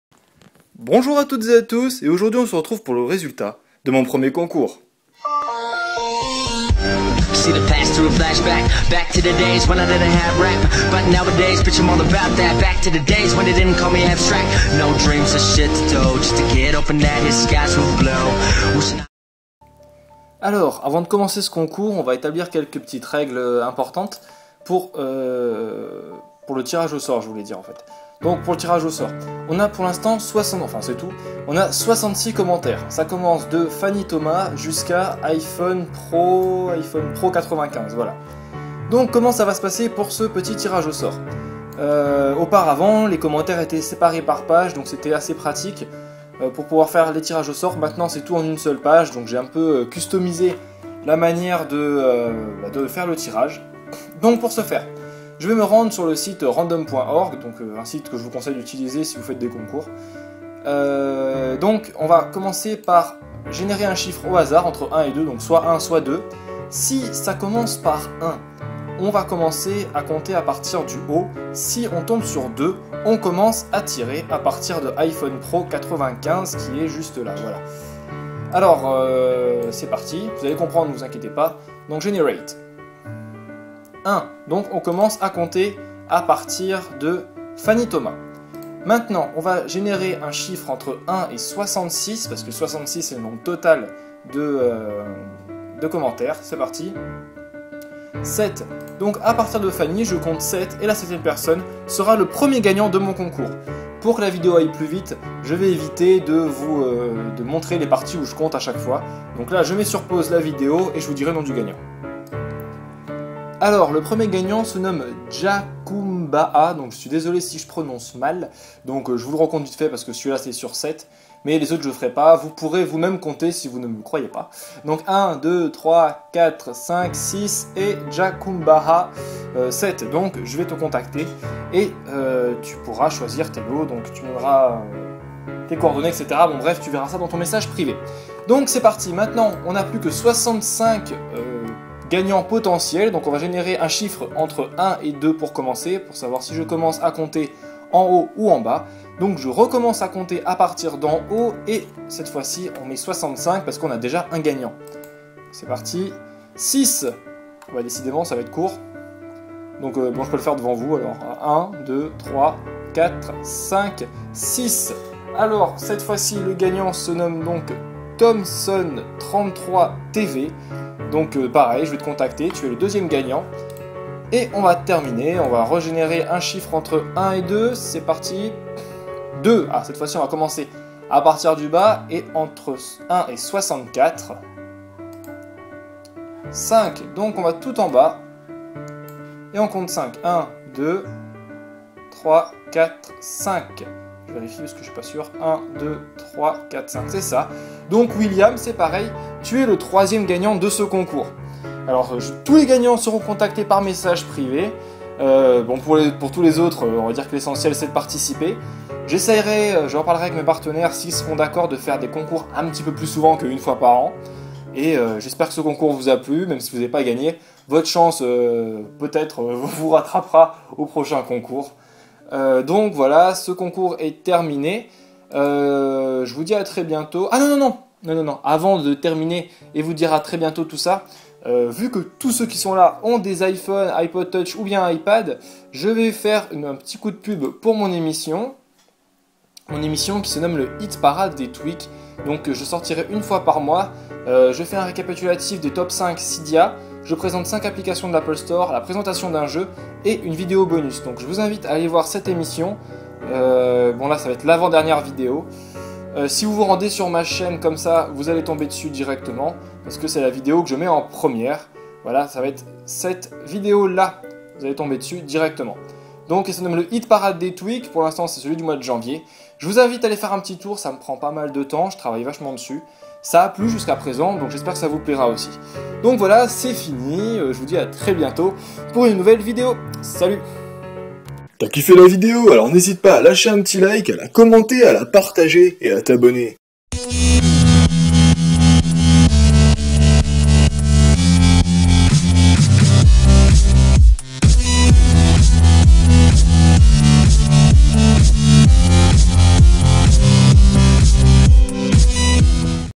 Résultat du concours 1 000 abonnées !!
Les gagnant recevront un message privé sur youtube d'ici peu.